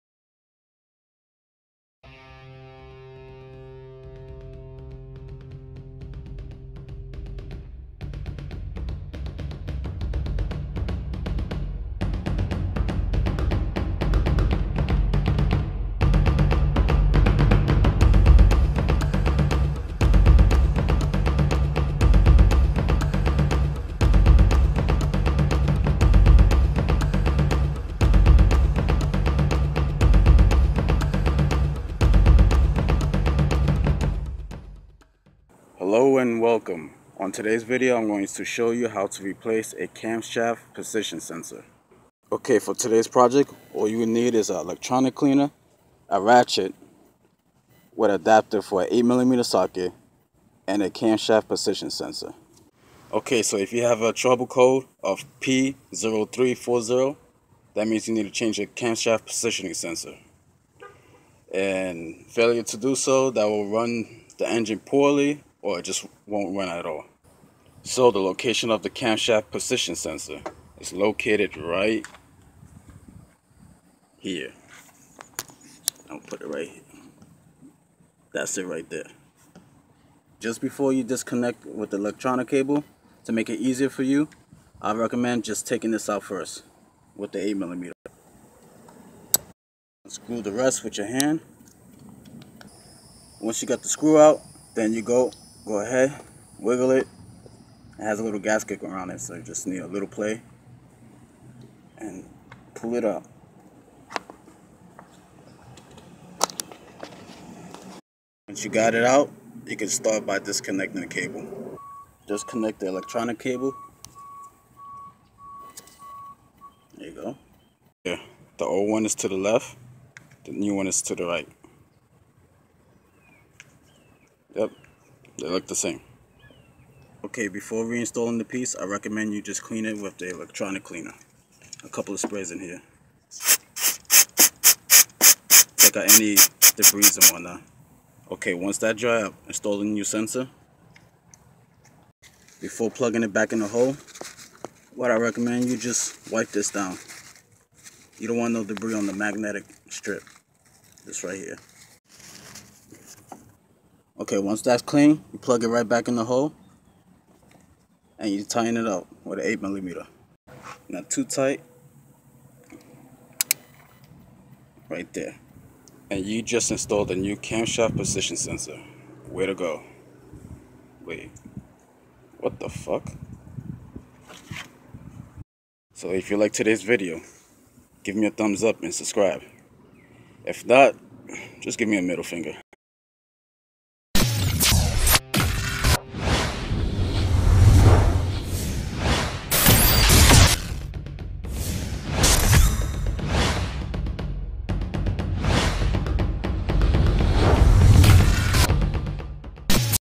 Cougar Repairs - Camshaft Position Sensor Replacement
If you have a error code P0340 then that means you have a Camshaft Position sensor (CMP)  Circuit Malfunction and needs to be replaced. Failure to do so will cause engine to run poorly or engine to not run at all.